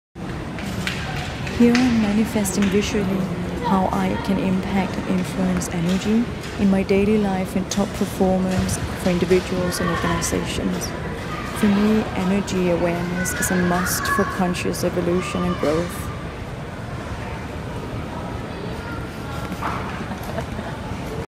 Christel Rosenkilde Christensen managing energy - Energistic Leadership Excellence™
Energy is manageable and a great tool for peak performance, happier lifestyles and positive impact.

Here I am manifesting visually how I can impact energy  - in my daily life optimizing top performance individually and for organizations.

Since I was a little girl I have seen and influenced the energy around me.

Later in life combining my academic background in business administration, leadership, negotiation, ethics and sustainability, my global business experience and my unique and strong ability to sense, see and impact energy I pioneered Energistic Leadership Excellence™ mentoring, training and advising top performers and organizations how to embrace a more conscious governance and awareness of energy in their leadership, performance and stakeholder chain - which create exceptional results, happier life style and a positive impact in the world we live in.

Be aware of how your thoughts, choices and actions impact the energy around you - it can be a 'make it or break it' I your business and private life.

Best of energy
Christel

Unfortunately the editing media cut away some of the screen and smoke... But I think you can get a good picture of how the energy move the smoke.